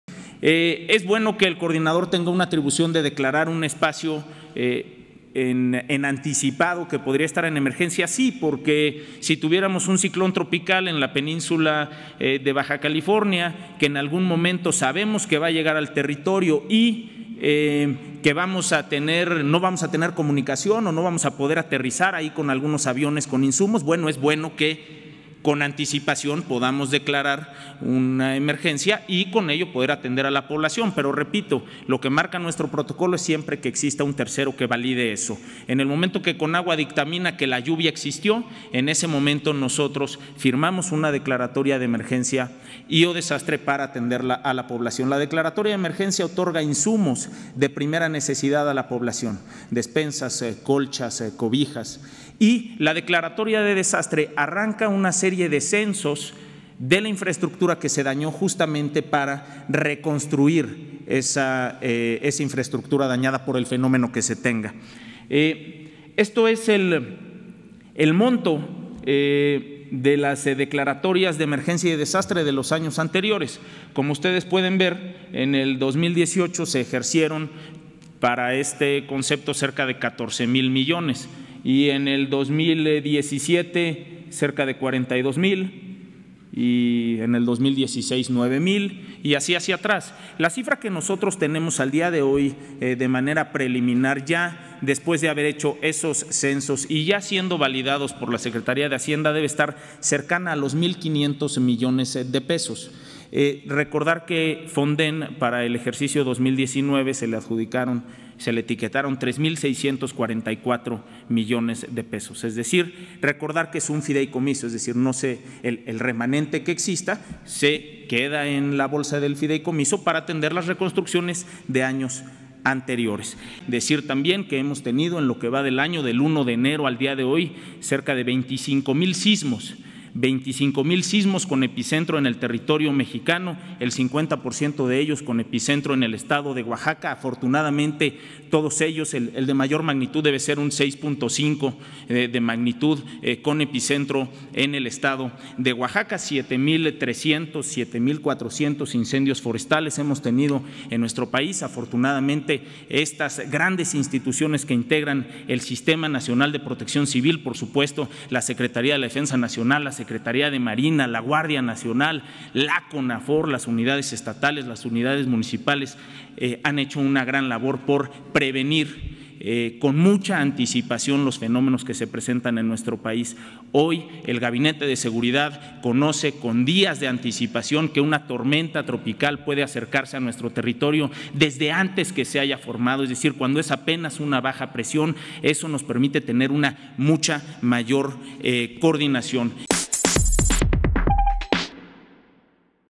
Van 25 mil sismos en lo que va del año
David León Romero, titular de la Coordinación Nacional de Protección Civil, informó que desde el 1 de enero, se han registrado 25 mil sismos con epicentro en el territorio nacional, 50% de ellos en el estado de Oaxaca.